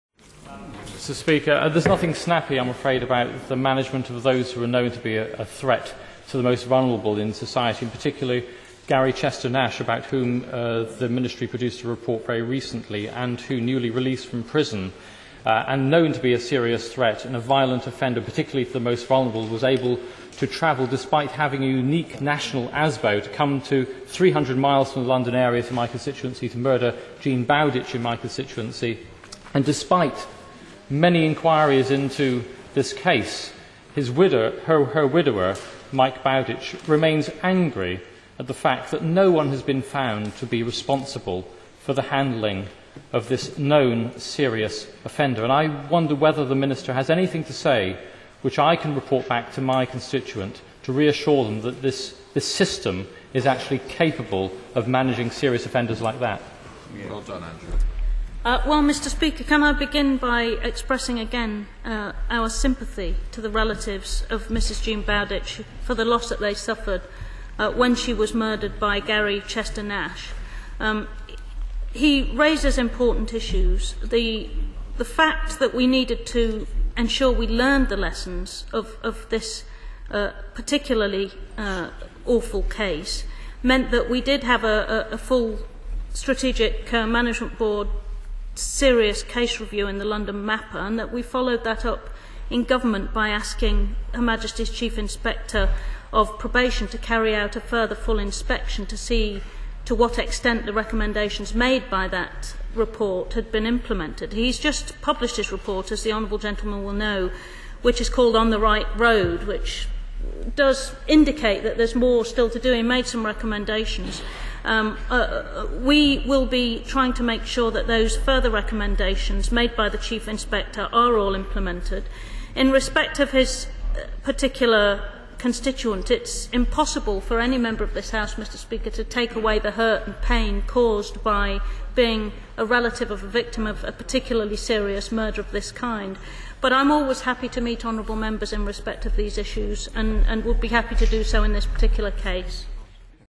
Andrew George MP Speaking in the House of Commons15th July 2008
Andrew George, MP for West Cornwall and Scilly constituency of St Ives speaking in the House of Commons